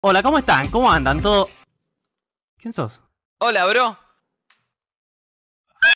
JUMPSCARE de PATO HORNEADO
Pato asado y atoj || Funny Moments || Momentos divertidos de pato horneado y atoj
Esta es una de las razones por las que amo a pato xd

Aqui el canal de pato Horneado (mi youtuber favorito :D)